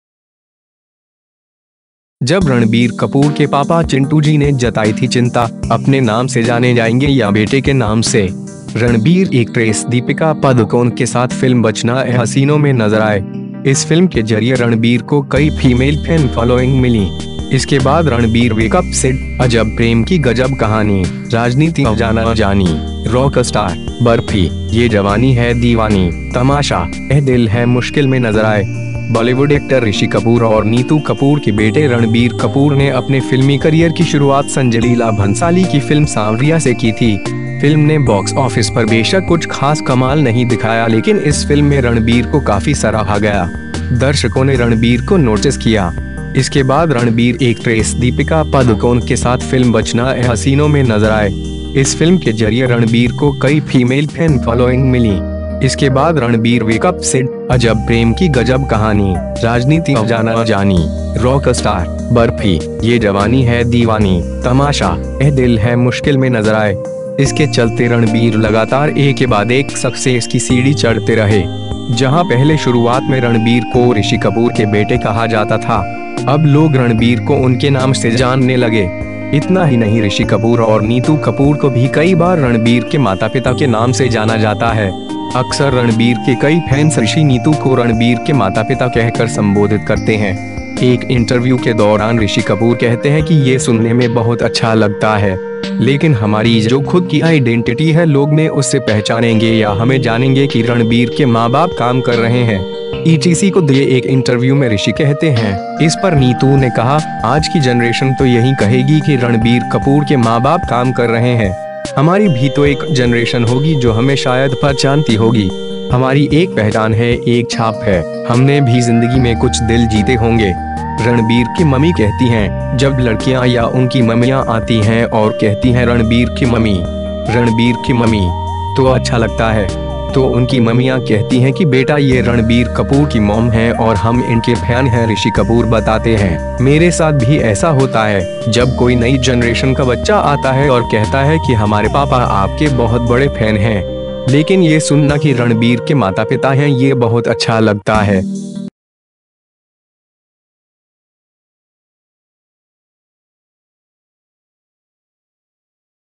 जब रणबीर कपूर के पापा ‘चिंटू जी’ ने जताई थी ‘चिंता’, अपने नाम से जाने जाएंगे या बेटे के नाम से!
रणबीर एक्ट्रेस दीपिका पादुकोण के साथ फिल्म 'बचना ऐ हसीनों' में नजर आए। इस फिल्म के जरिए रणबीर को कई फीमेल फैंन फॉलोइंग मिली। इसके बाद रणबीर वेकअप सिड, अजब प्रेम की गजब कहानी, राजनीति अंजाना अंजानी, रॉकस्टार, बर्फी, ये जवानी है दीवानी, तमाशा, ऐ दिल है मुश्किल में नजर आए।

बॉलीवुड एक्टर ऋषि कपूर और नीतू कपूर के बेटे रणबीर कपूर ने अपने फिल्मी करियर की शुरुआत संजय लीला भंसाली की फिल्म ‘सांवरिया’ से की थी। फिल्म ने बॉक्स ऑफिस पर बेशक कुछ खास कमाल नहीं दिखाया लेकिन इस फिल्म में रणबीर को काफी सराहा गया। दर्शकों ने रणबीर को नोटिस किया। इसके बाद रणबीर एक्ट्रेस दीपिका पादुकोण के साथ फिल्म ‘बचना ऐ हसीनों’ में नजर आए। इस फिल्म के जरिए रणबीर को कई फीमेल फैंन फॉलोइंग मिली। इसके बाद रणबीर वेकअप सिड, अजब प्रेम की गजब कहानी, राजनीति अंजाना अंजानी, रॉकस्टार, बर्फी, ये जवानी है दीवानी, तमाशा, ऐ दिल है मुश्किल में नजर आए।

इसके चलते रणबीर लगातार एक के बाद एक सक्सेस की सीढ़ी चढ़ते रहे। जहां पहले शुरुआत में रणबीर को ऋषि कपूर के बेटे कहा जाता था। अब लोग रणबीर को उनके नाम से जानने लगे। इतना ही नहीं ऋषि कपूर और नीतू कपूर को भी कई बार रणबीर के माता पिता के नाम से जाना जाता है। अक्सर रणबीर के कई फैंस ऋषि-नीतू को ‘रणबीर के माता-पिता’ कहकर संबोधित करते हैं।

एक इंटरव्यू के दौरान ऋषि कपूर कहते हैं कि ये सुनने में बहुत अच्छा लगता है। लेकिन हमारी जो खुद की आईडेंटिटी है लोग में उससे पहचानेंगे या हमें जानेंगे कि रणबीर के मां बाप काम कर रहे हैं। ईटीसी को दिए एक इंटरव्यू में ऋषि कहते हैं, ‘ इस पर नीतू ने कहा, आज की जनरेशन तो यही कहेगी कि रणबीर कपूर के मां-बाप काम कर रहे हैं। हमारी भी तो एक जनरेशन होगी जो हमें शायद पहचानती होगी। हमारी एक पहचान है एक छाप है, हमने भी जिंदगी में कुछ दिल जीते होंगे।’

रणबीर की मम्मी कहती हैं, ‘जब लड़कियां या उनकी मम्मियां आती हैं और कहती हैं रणबीर की मम्मी.. रणबीर की मम्मी.. तो अच्छा लगता है। तो उनकी मम्मियां कहती हैं कि बेटा ये रणबीर कपूर की मॉम हैं और हम इनके फैन हैं।’ ऋषि कपूर बताते हैं, ‘मेरे साथ भी ऐसा होता है, जब कोई नई जनरेशन का बच्चा आता है और कहता है कि हमारे पापा आपके बहुत बड़े फैन हैं। लेकिन ये सुनना कि रणबीर के माता पिता हैं- ये बहुत अच्छा लगता है।’